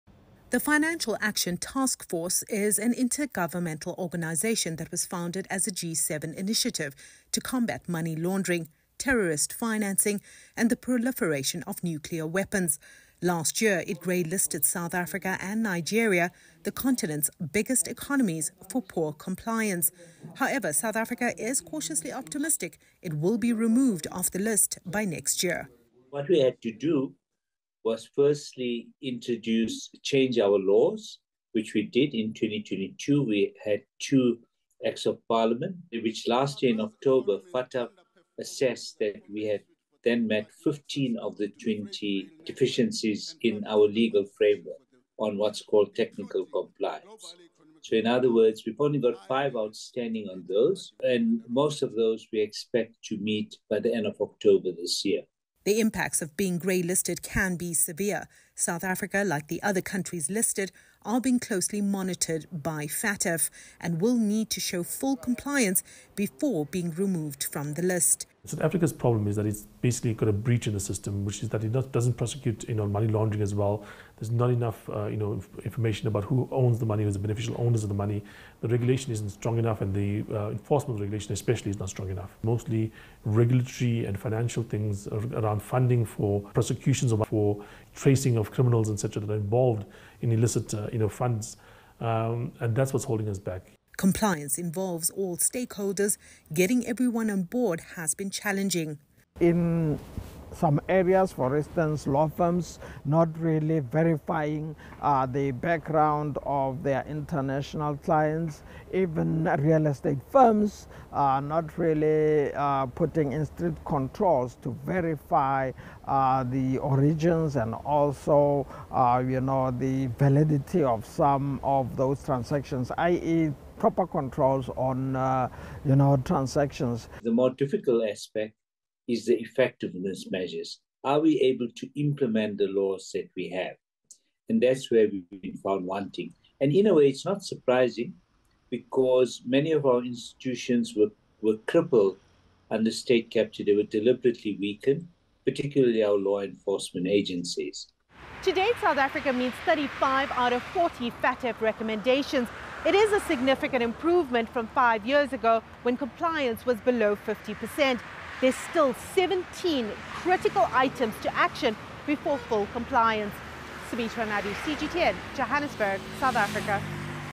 South Africa optimistic it will be removed from Financial Action Task Force grey list
Policymakers in the country are making progress in strengthening legislation to meet key requirements of the international watchdog. The Financial Action Task Force put the country on its list in February 2023 for falling short of certain international standards for the combating of money laundering and other serious financial crimes.